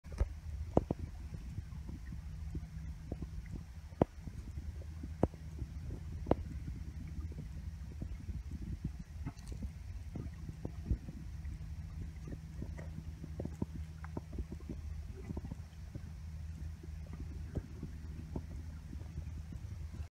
Kingsnake goes for a swim - Aliso & Woods Canyon
Kingsnake trips on leaf and swims away in embarrassment.